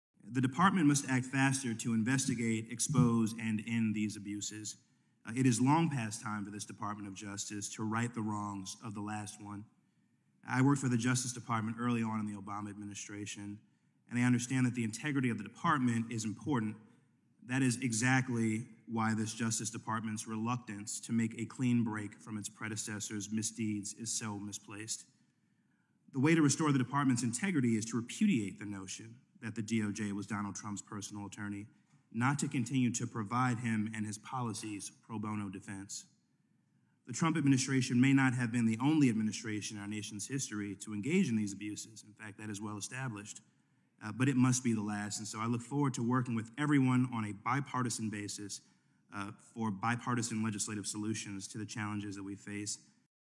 Mondaire Jones URGES DOJ to work faster on Trump case
In a house hearing statemen, Rep. Mondaire Jones urged DOJ to work faster on the Apple emails case.